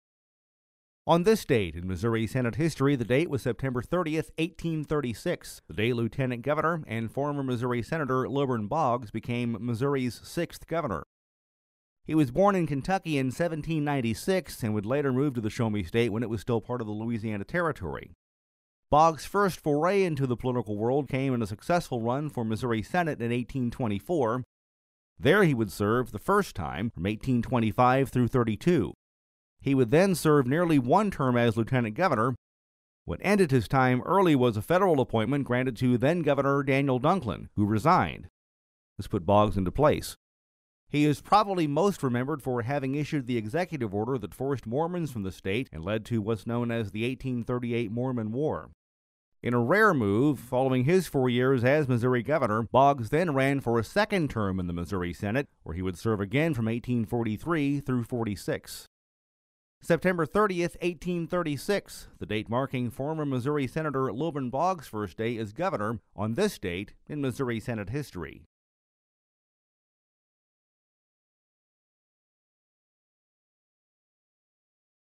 This Date in Missouri Senate History: Sept. 30, 1836
JEFFERSON CITY — This Date in Missouri Senate History reviews Sept. 30, 1836, the day Lt. Gov. — and former Missouri Sen. — Lilburn Boggs became Missouri’s sixth governor.